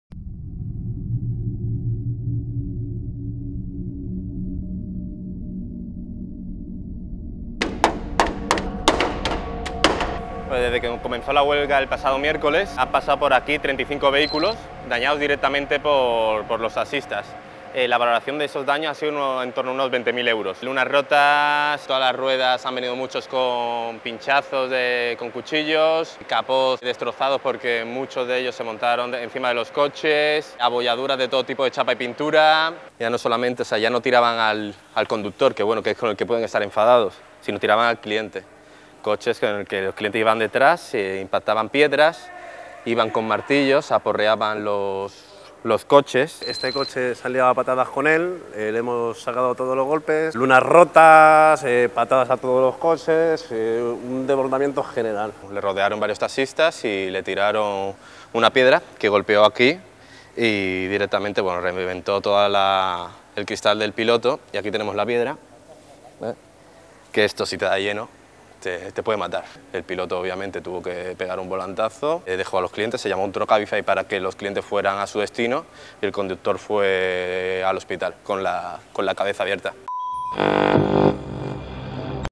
El museo de los horrores de las VTC
Visitamos los talleres de Auro New Transport Concept, cuya flota de vehículos se ha visto seriamente afectada por la violencia ejercida por los taxistas en la huelga que han protagonizado.